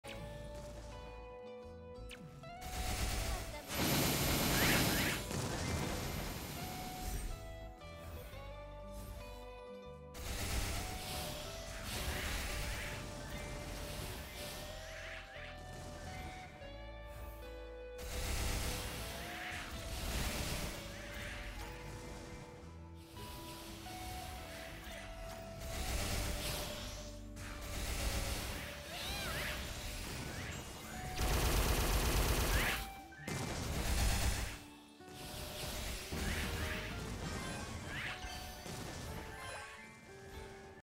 264 to 270 Bishop in a week, Loot room videos back soon
PC got fixed, waiting for SAC gate so trained a ton on my bsp